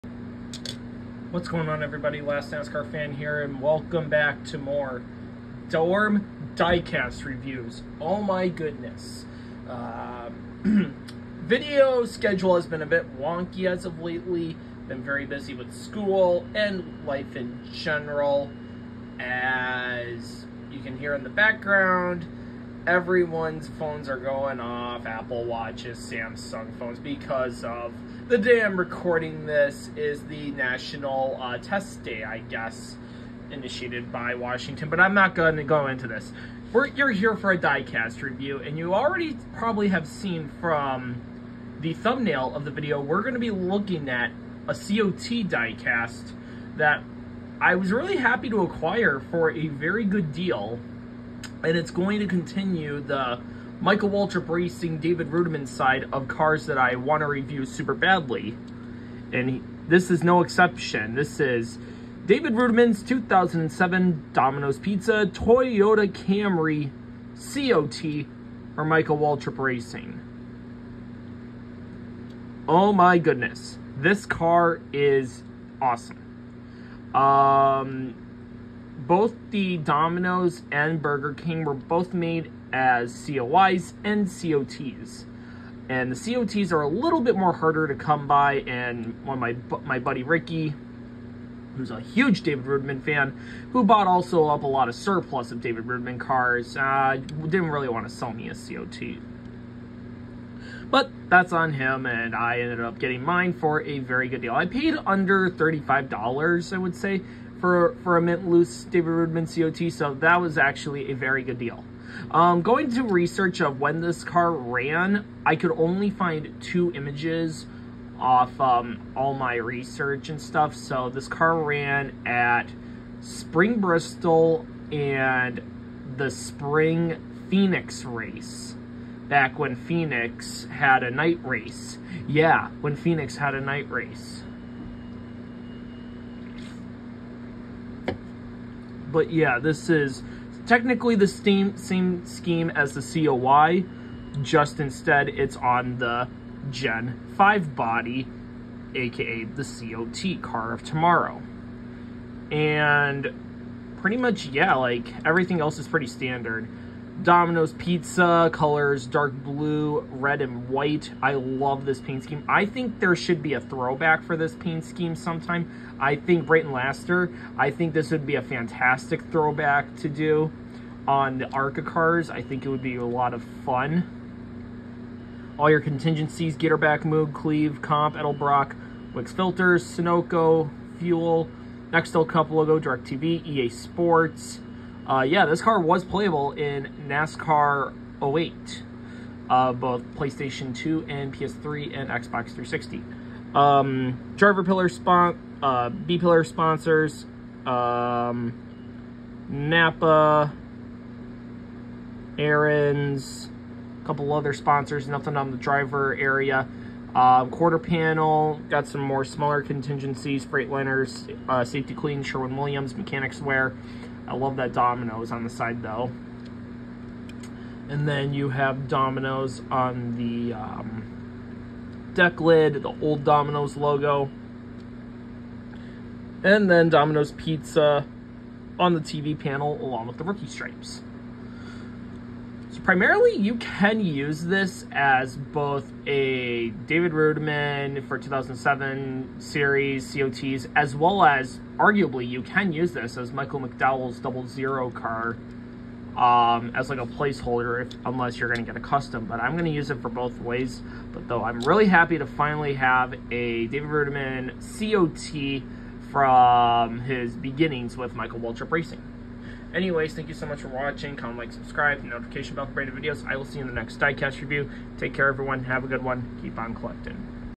2007 David Reutimann #00 Dominos Pizza (COT) 1:64 Diecast Review
After years of trying to obtain this promo I was able to snag this off eBay for a steal.

Only photos I could find this scheme was Spring Bristol and Spring Phoenix. If anyone knows the other races where it ran lmk.

This is not kid friendly you’re welcome